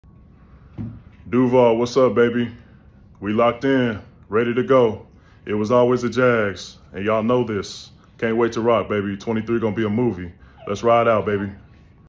Roy Robertson-Harris is locked in 📝 💯
DL Roy Robertson-Harris signs a contract extension ahead of the 2023 Season.